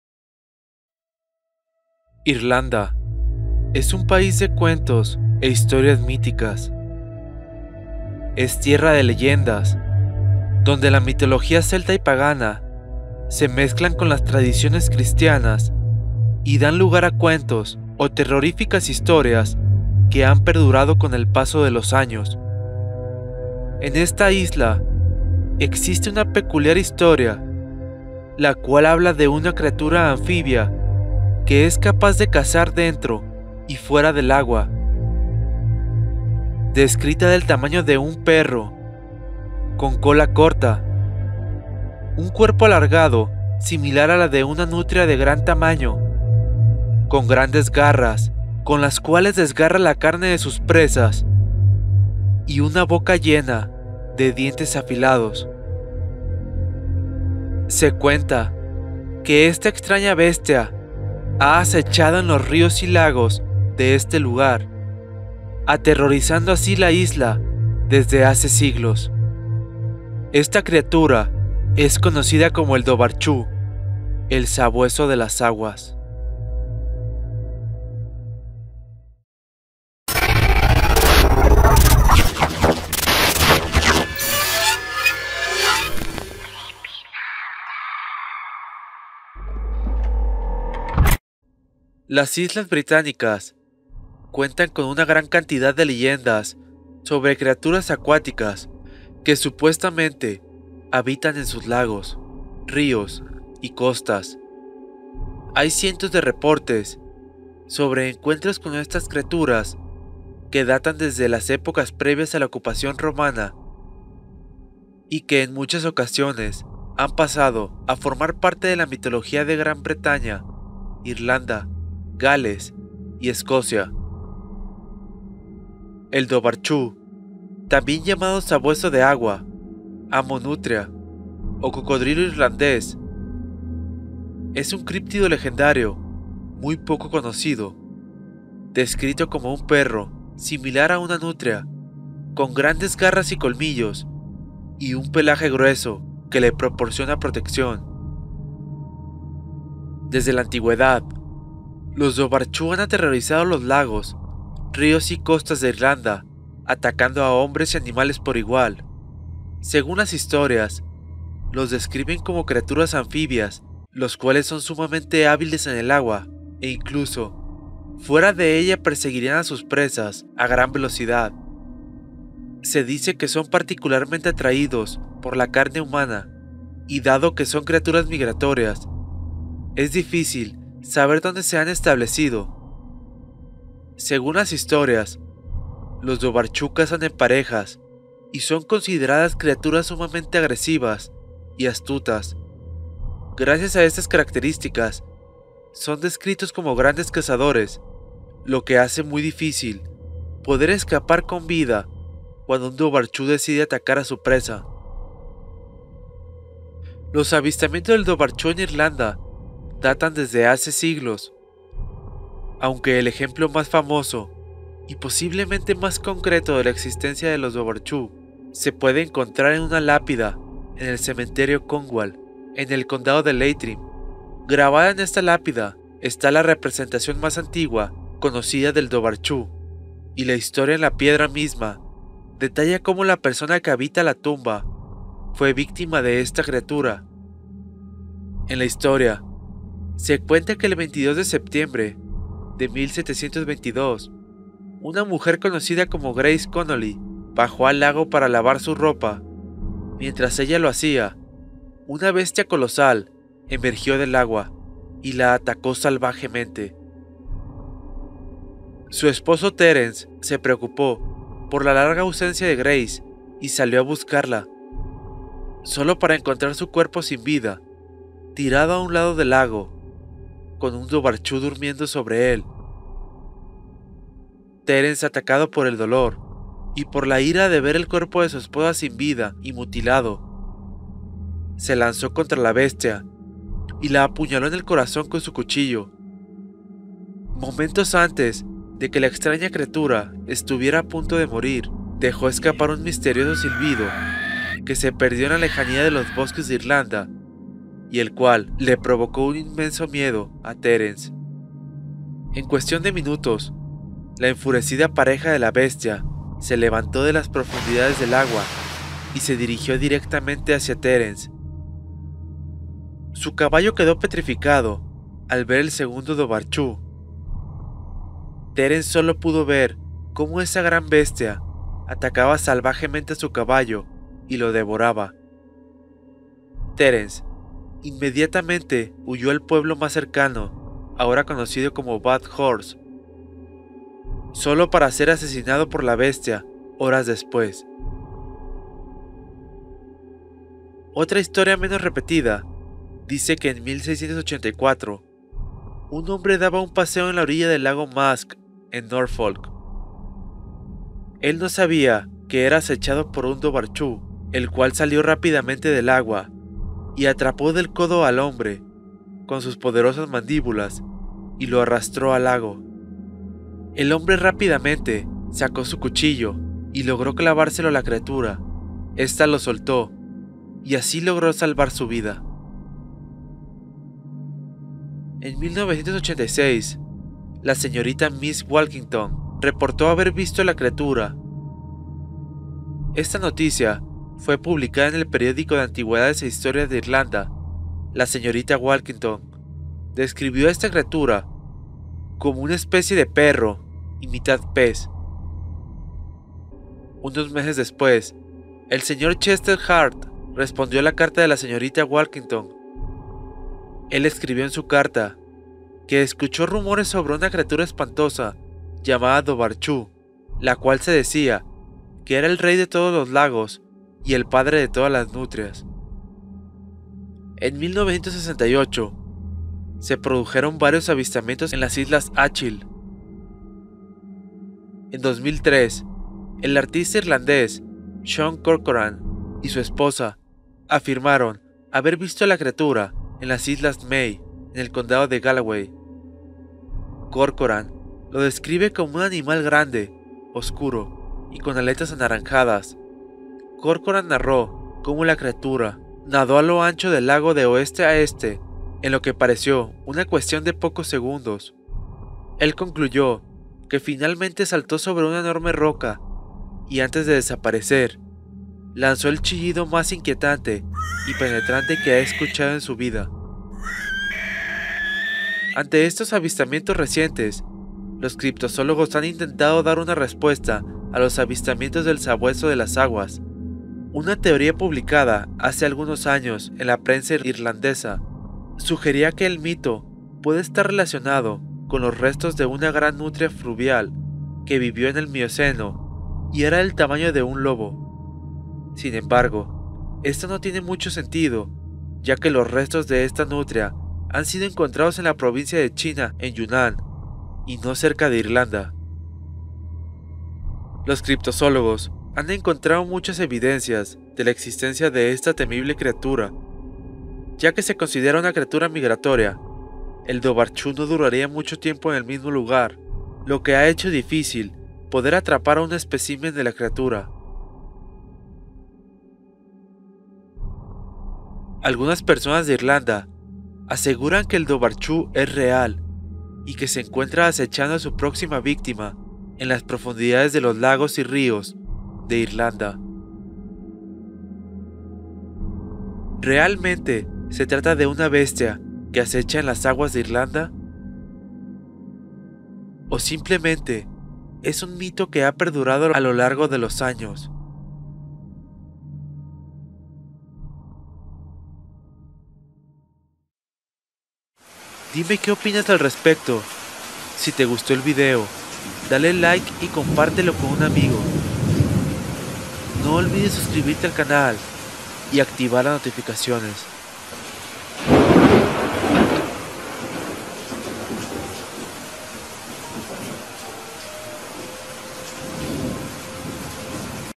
Bestiario: DOBHAR CHU - El Sabueso de las Aguas Irlandesas | Monstruo Acuático | CRIPTOZOOLOGIA
EL DOBHAR CHU O SABUESOS DE LAS AGUAS ES UN CRIPTIDO QUE ASECHA EN LAOS RIOS Y LAGOS DE IRLAND , SUS AVISTAMIENTOS DATAN DESDE HACE CIENTOS DE AÑO.